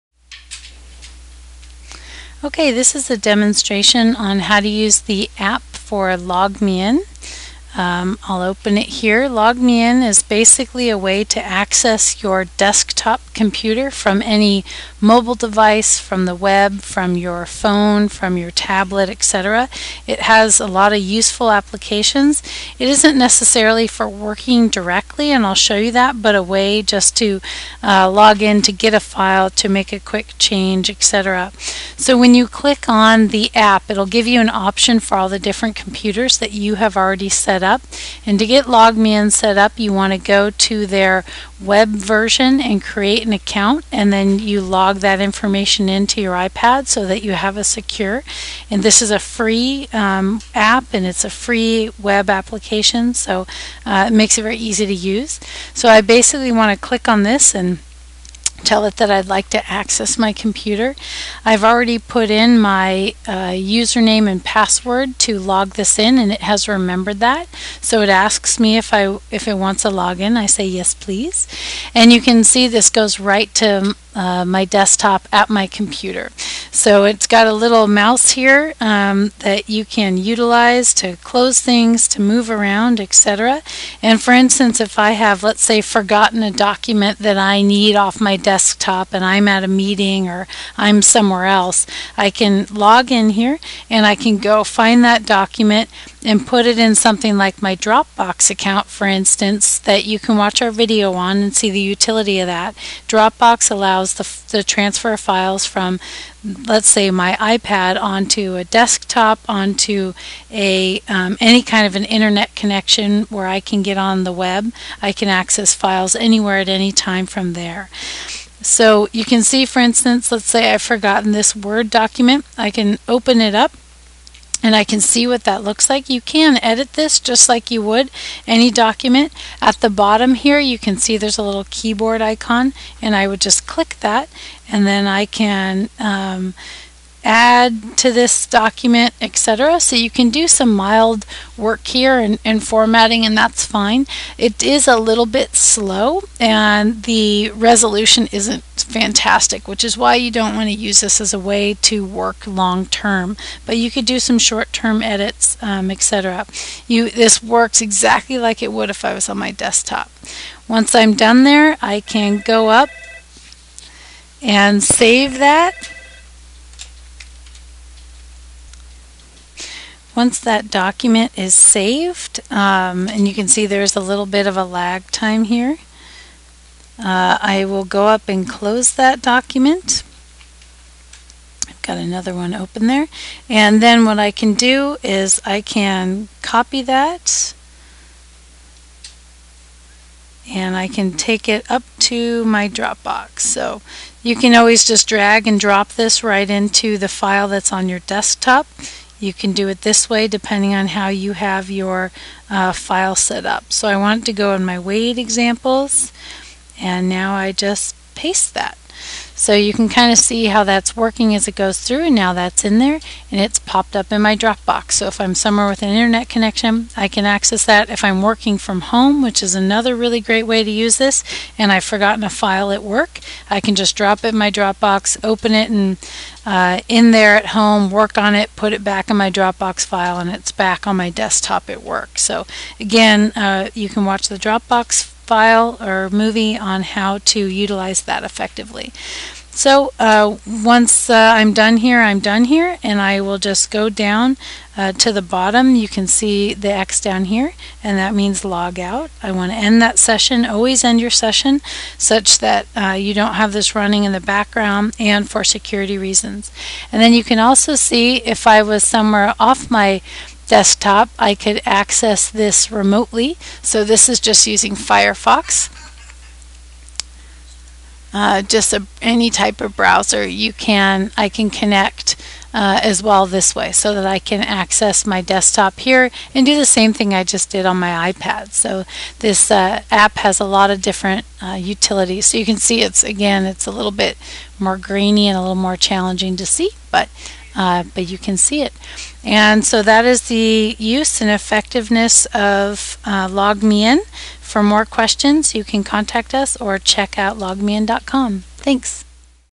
Using LogMeIn on the iPad for Conservation Planning
Demonstrating the use of LogMeIn on the iPad for Conservation Planning.